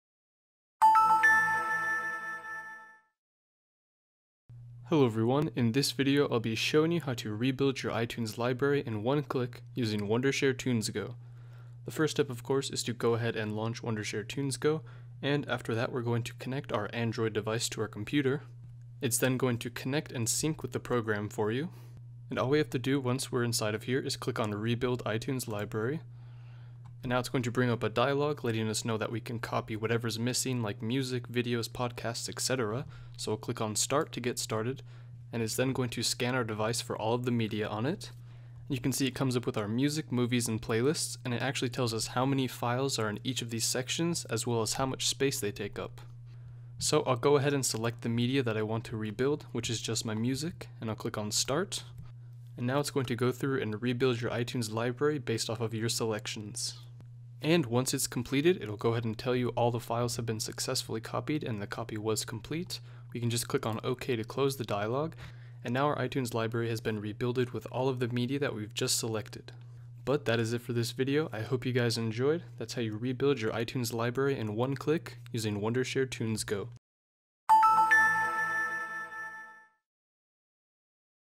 1-Click Rebuild iTunes Library with Android
The tutorial on how to rebuild iTunes library quickly and how to transfer music, video files from Android phone to iTunes Library with TunesGo